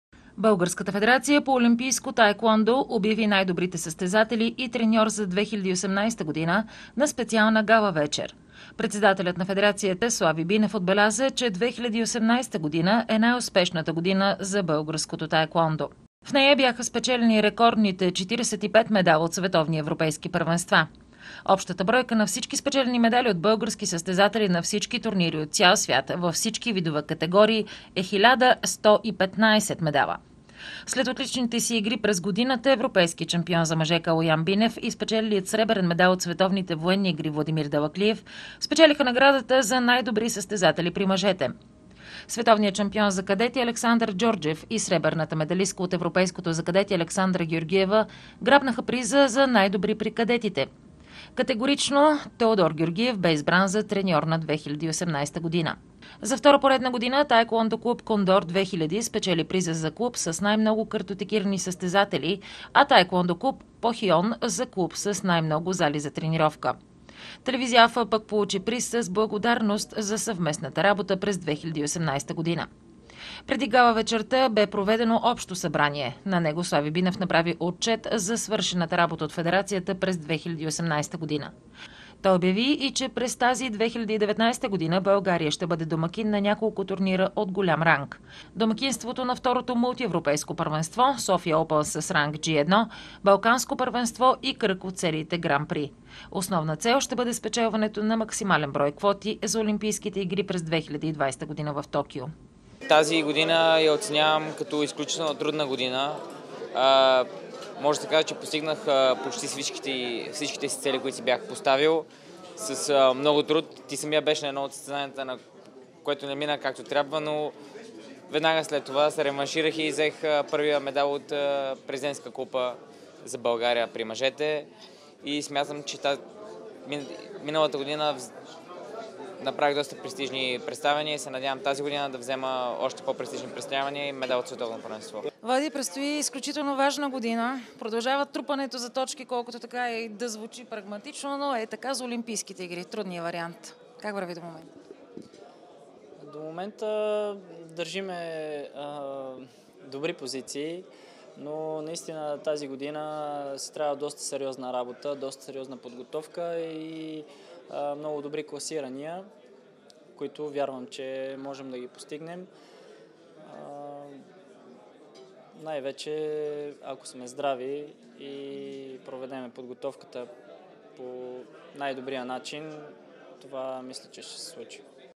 Олимпийското ни таекуондо обяви най-добрите за 2018 година /21.01.2019 г./
Според председателя на федерацията Слави Бинев изминалата година е най-успешната за българското таекуондо. През 2018-а са спечелени рекордните 45 медала от световни и европейски първенства